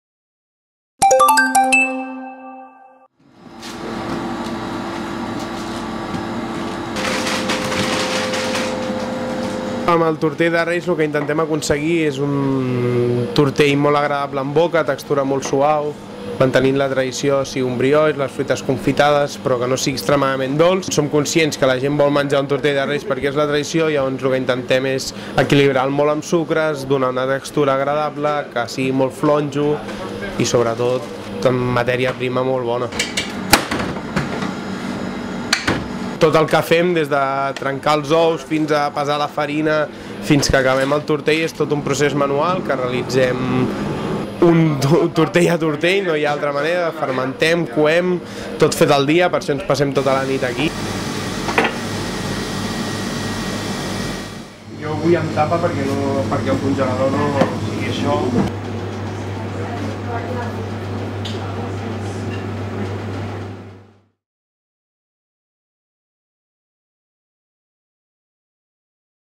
Els pastissers, a tota màquina per tenir a punt els tortells de Reis
Visitem la pastisseria Cal Jan de Torredembarra per veure-ho.